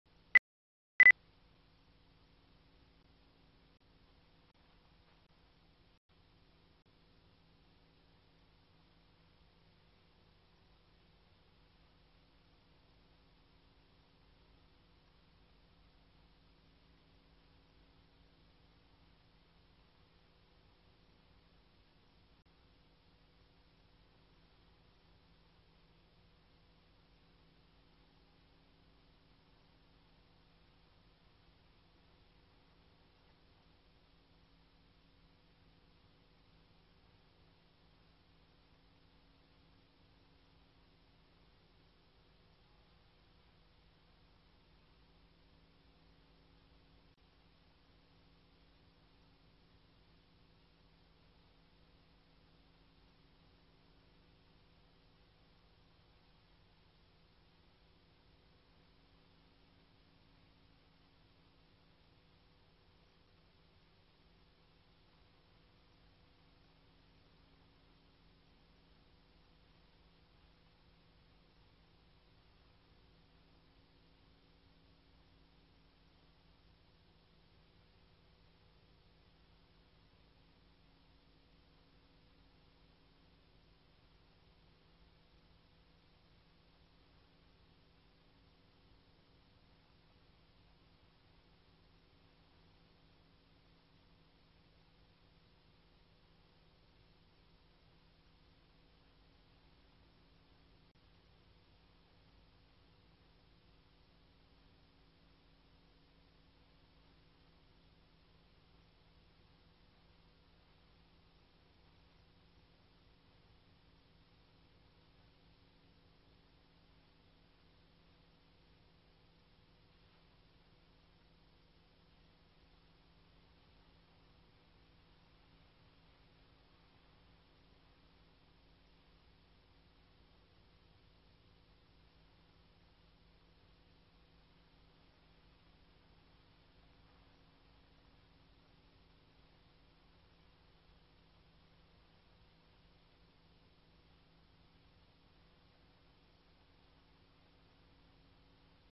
Indy enjoying the windy morning.flv
AEF's eagle Independence enjoying the windy morning